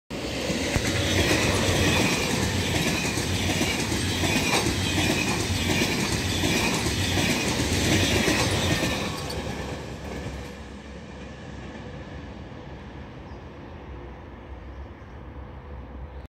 TGV Bas Carbone sul Milano-Parigi! - TGV Bas Carbone sur le Milan-Paris!
La livrea "Grand vitesse, bas carbone" è arrivata anche sui TGV Milano-Parigi! Il treno in questione è il TGV-R 4501. Buona visione!